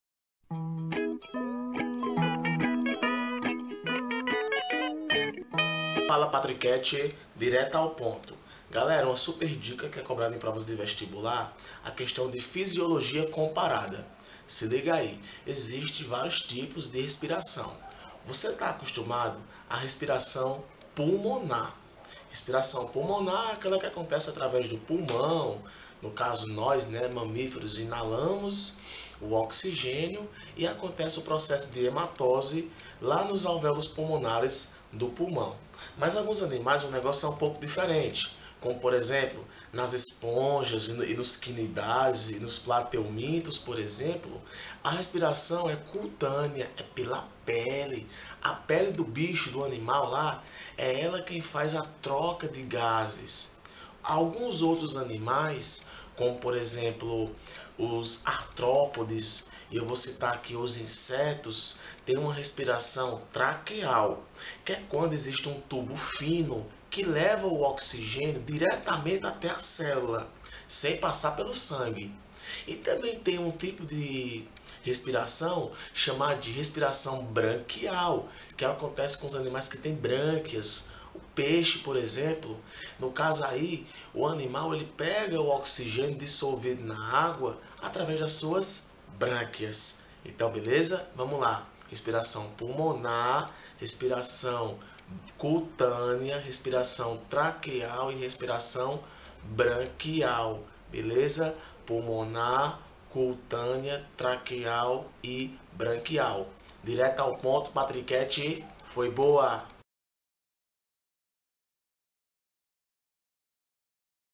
Aula 8: Sistema Respiratório: Tipos de Respiração - Ensino Médio - Biologia com Patrick Gomes
Aula 8: Sistema Respiratório: Tipos de Respiração - Aula de Biologia Ensino Médio e Vestibulares
Entenda de vez os Tipos de Respiração presente no Sistema Respiratório de diferentes tipos de animais.  Biologia com Patrick Gomes
▶ CONVITES PARA PALESTRAS, AULAS OU EVENTOS!

Sinopse do vídeo:
É através do processo respiratório que os animais conseguem fazer as trocas gasosas com o meio ambiente. Através da respiração aeróbica, os animais obtém oxigênio e eliminam o gás carbônico no meio externo.